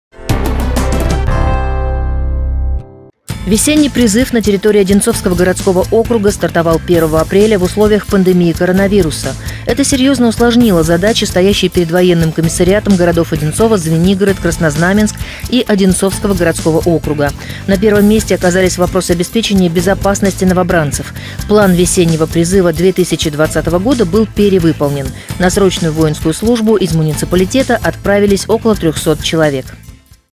Итоги призыва
Весенний призыв на территории Одинцовского городского округа стартовал 1 апреля, в условиях пандемии коронавируса.